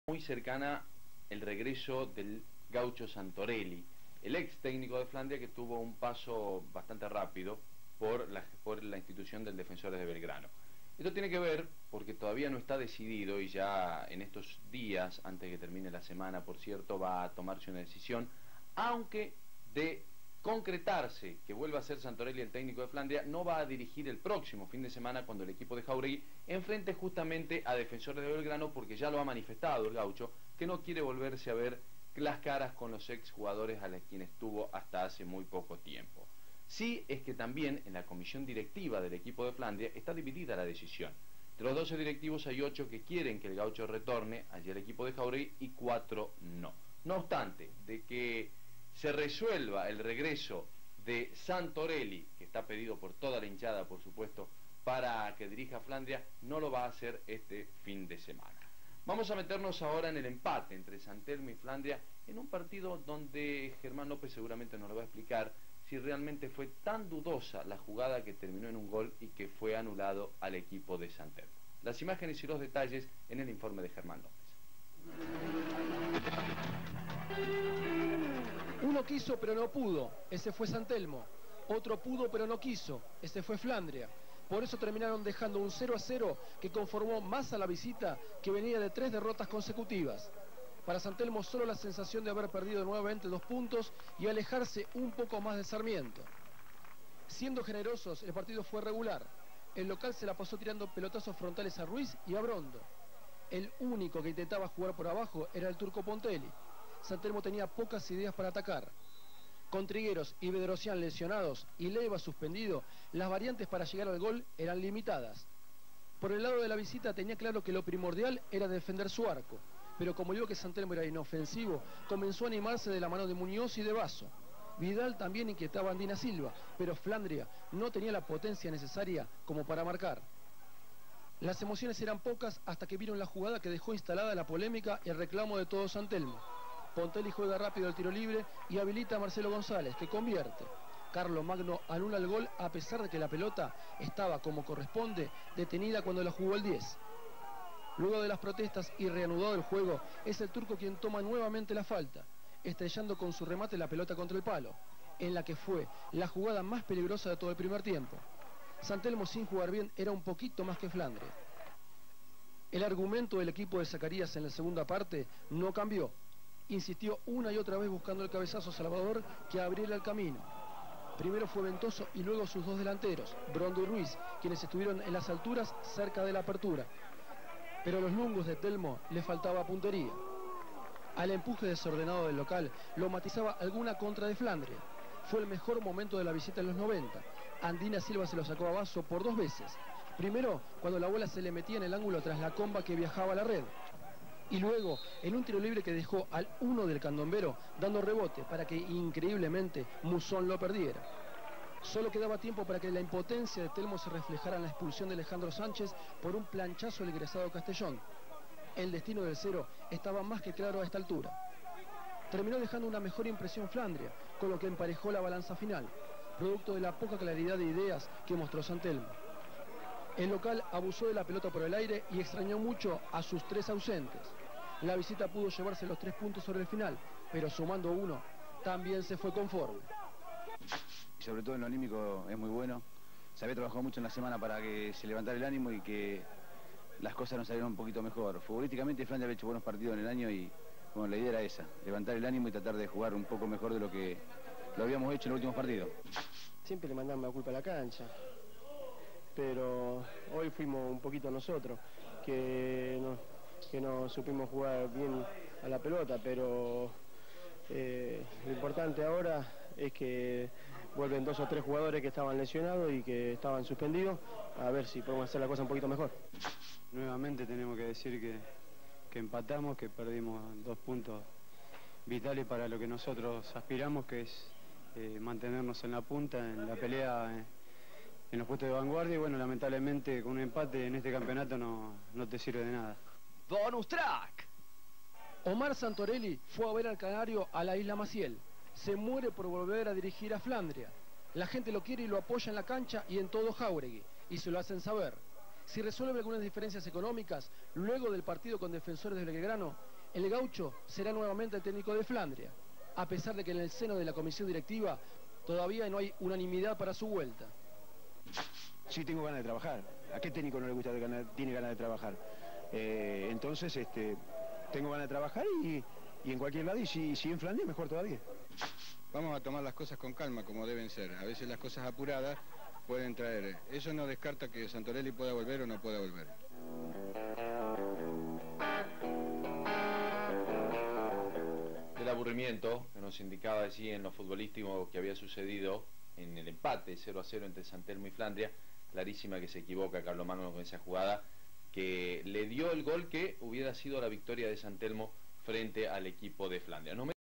San Telmo 0 - 0 Flandria. Temporada 2000/01. Resumen del partido por ABCDiario.
Empate en la Isla Maciel, con Geeardo "Piri" Garate como DT interino.
Declaraciones de Santorelli (candidato a DT) y del Dr. Hernán Mosca (Presidente).

Partido disputado el 25/11/2000.